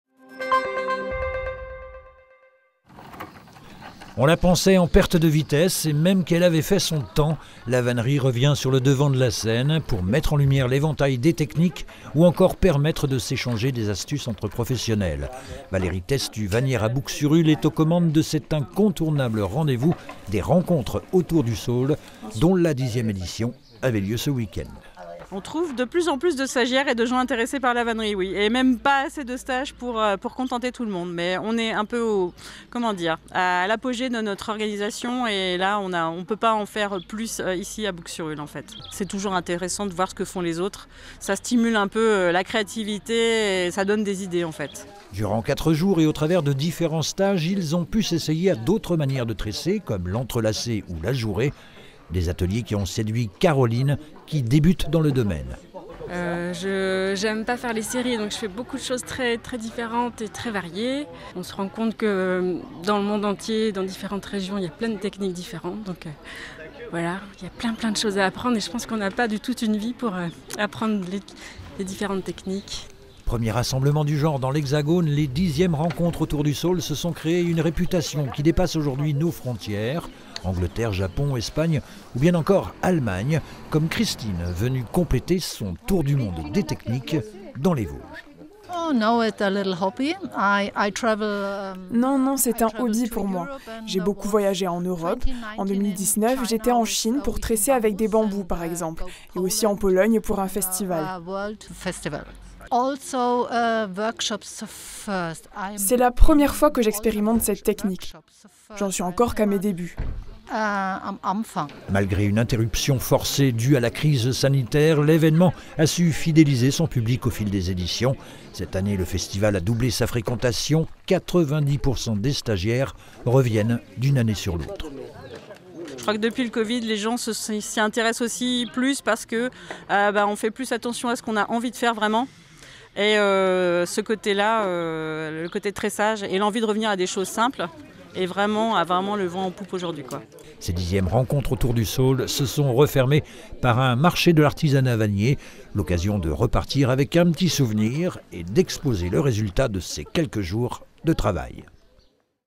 REPORTAGE - Vannerie: et si vous osier(z) ?
Les 10èmes rencontres autour du saule, premier festival du genre en France mais également reconnu à l'international, permettent aux professionnels mais aussi aux amateurs d'échanger autour de leurs techniques et de partager leurs savoir-faire. Une centaine de stagiaires se sont ainsi pris au jeu durant quelques jours pour (ré)apprendre à confectionner des paniers selon différentes méthodes du monde.
Images: F. SALETTI
Texte: T. HERMANN-QUESADA
Montage: T. HERMANN-QUESADA
Commentaire: T. LASSON